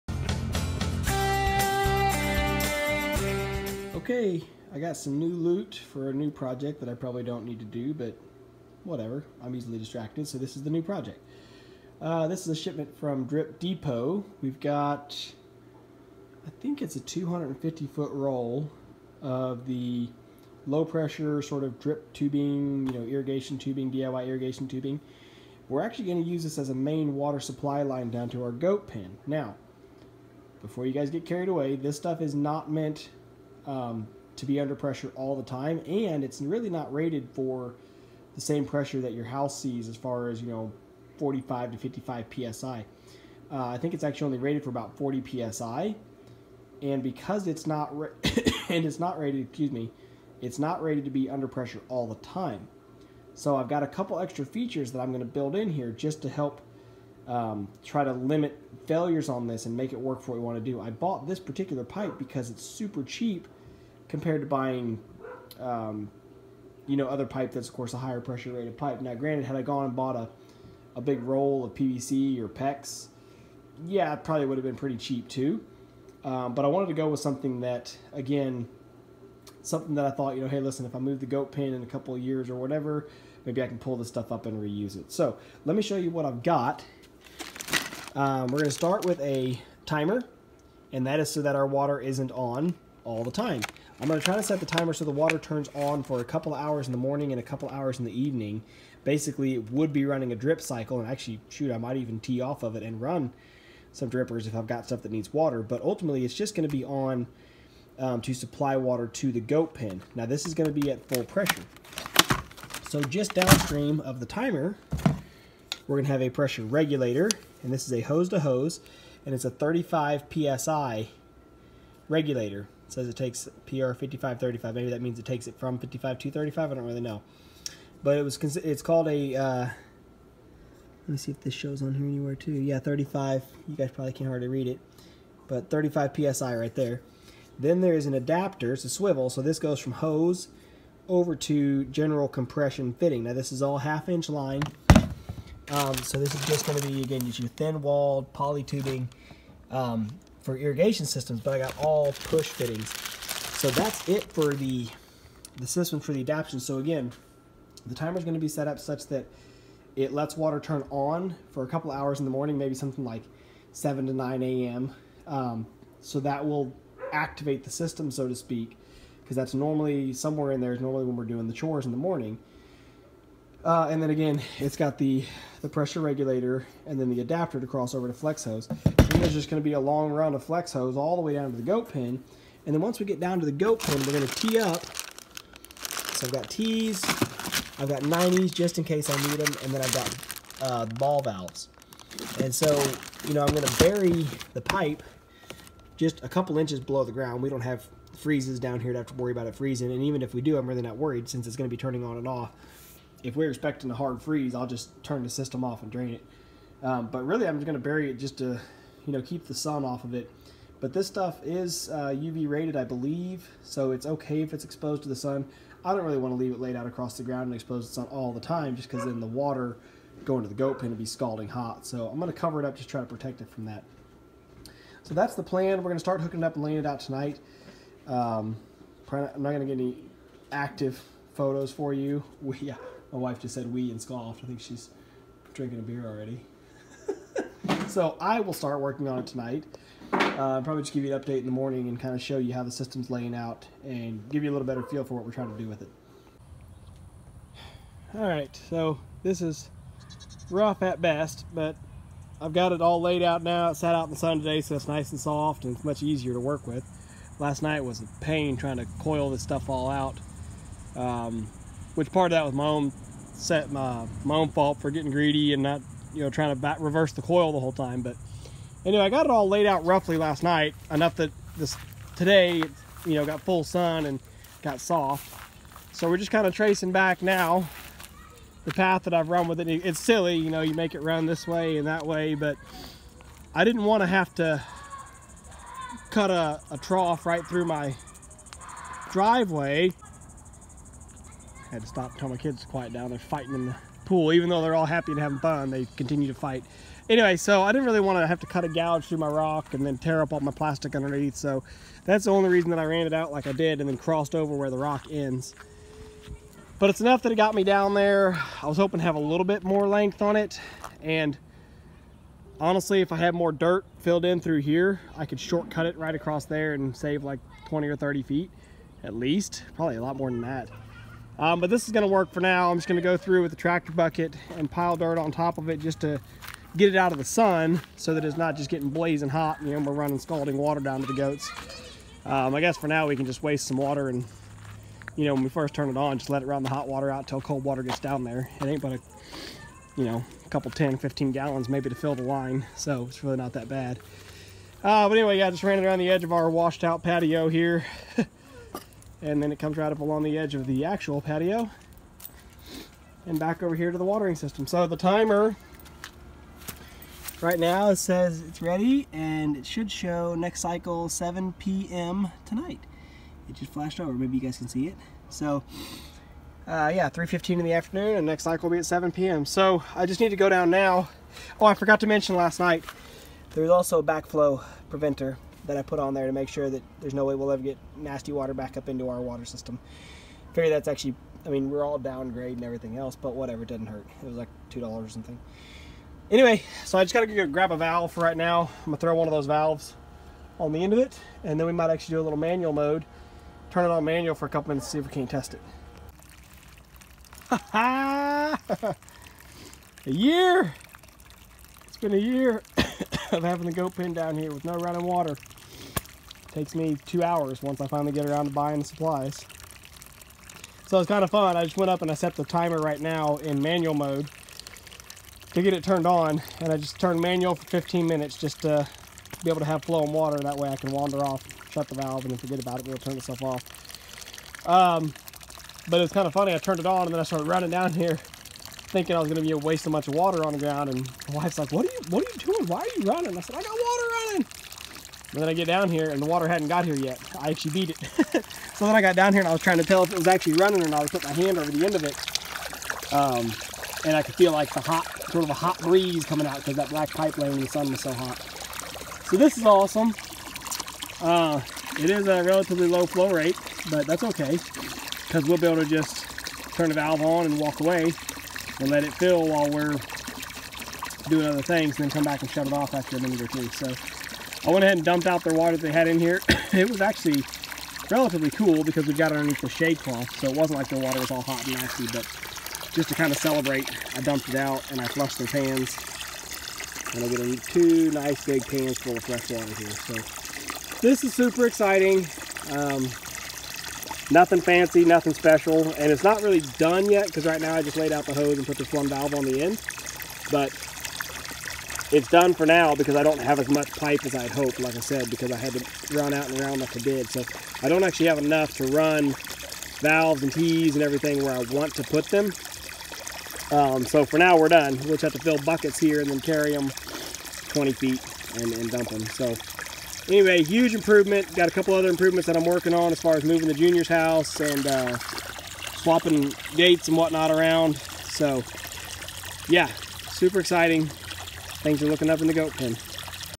Installing Running Water to the Goat Pen
My Standard Workware:
Leather Gloves
Muck Boots

It’s been over a year that we’ve had the goats. And a LOT of buckets of water carried down. Finally! Finally I’m installing running water. And it’s such a relief!